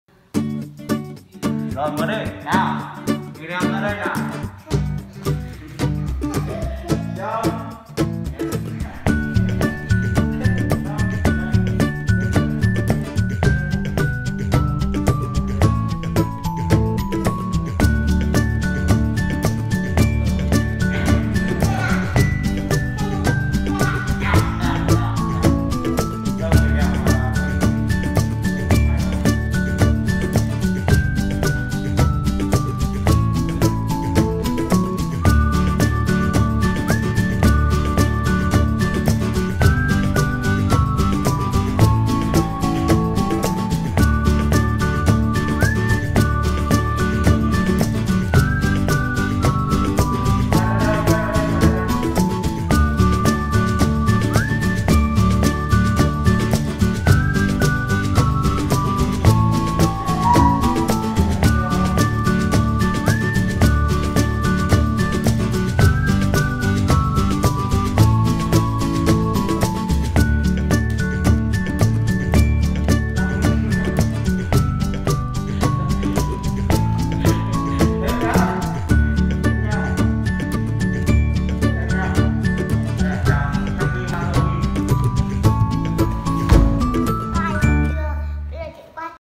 Halloween dance
Viet Nam person's A extempore figure-dance during the night halloween.